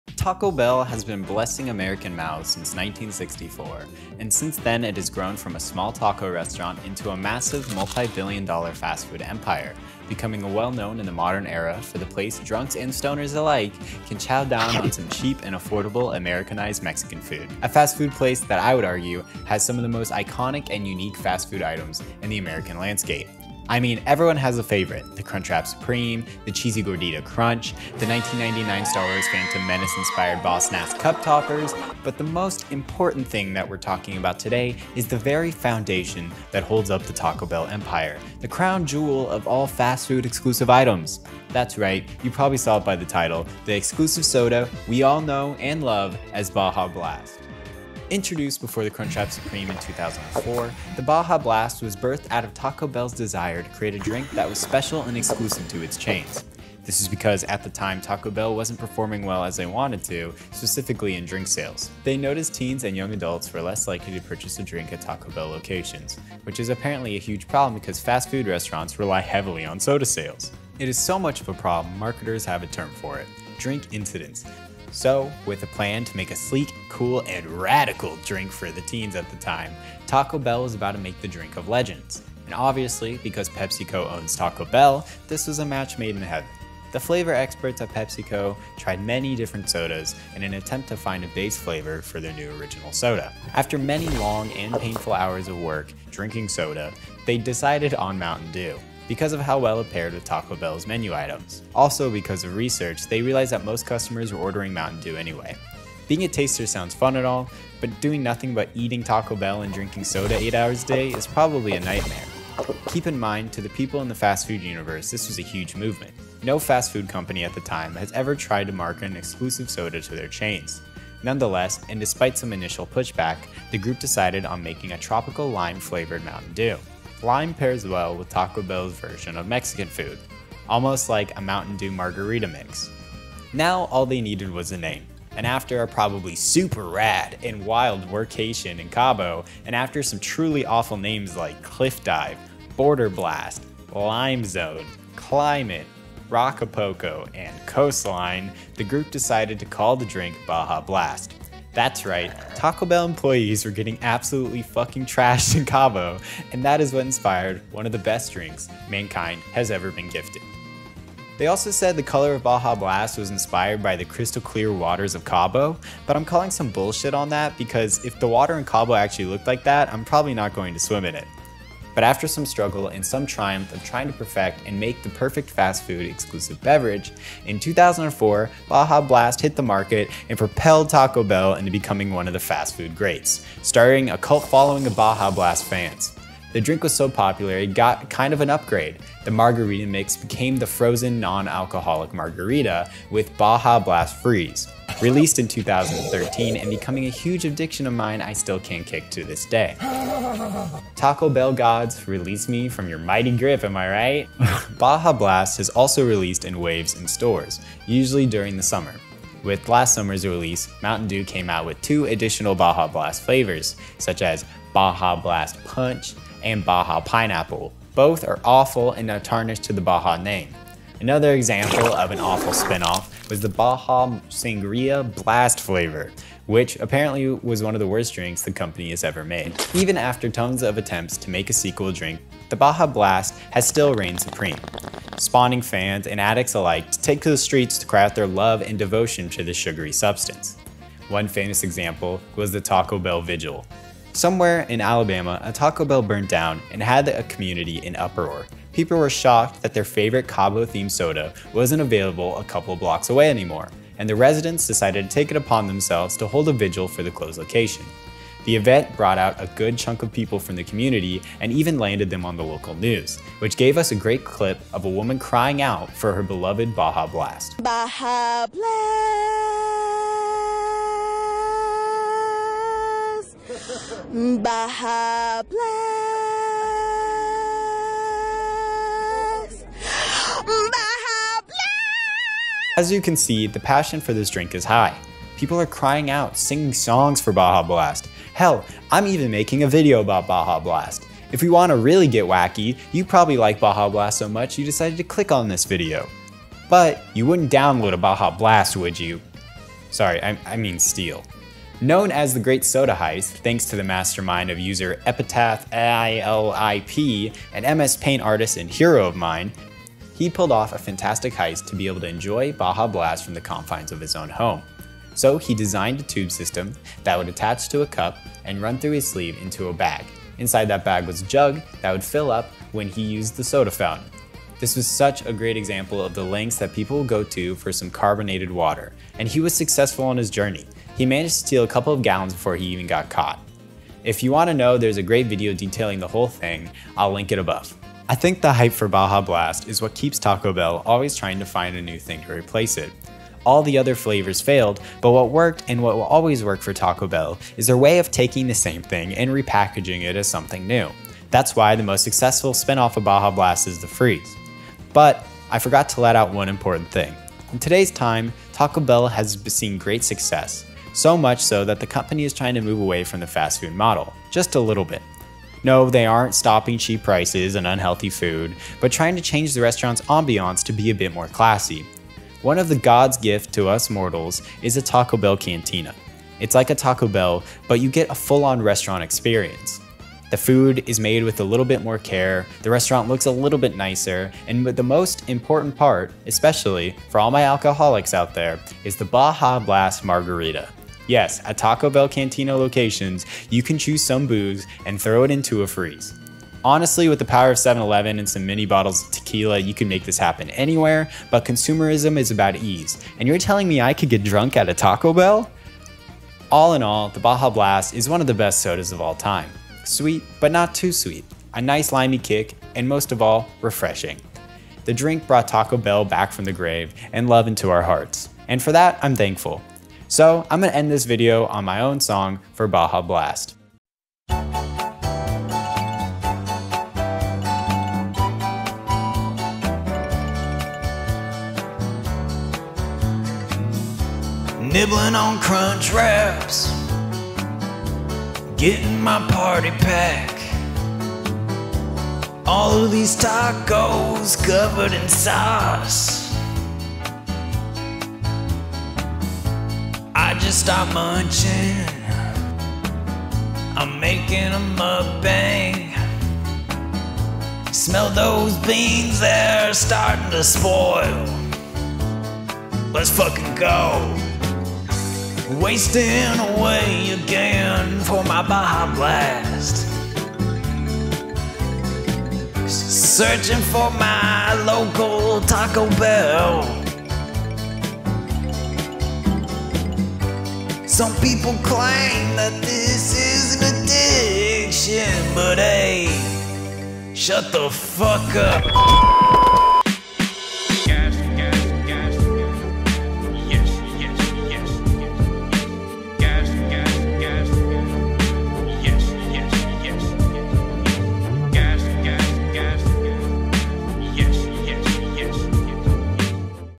The REAL History of Baja Blast
Taco Bell has been blessing American mouths since 1964. And since then it has grown from a small taco restaurant into a massive multi billion dollar fast food empire. When times got rough The company turned too a new and revolutionary soda to save the day.

Oh man I love Baja Blast so much it's so damn tasty holy smokes!

Everything and nothing is a show where each week we discuss a niche topic. From Everything to nothing we cover it.